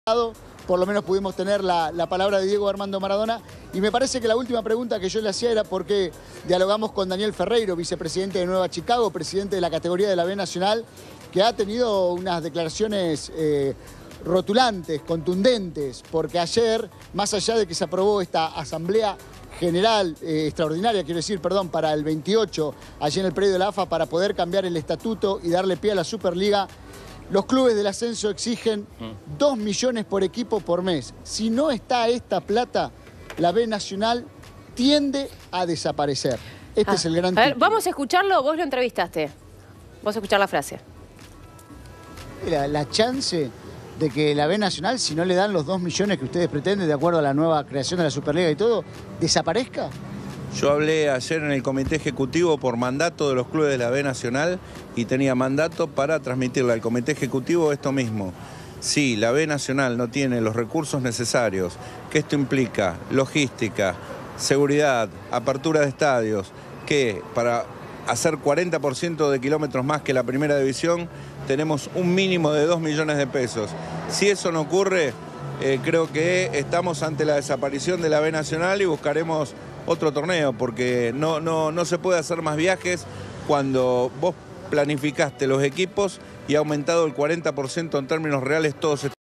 El Nacional B puede desaparecer
Los dirigentes de dicha categoría expresaron que no pueden aceptar menos del dinero que pidieron.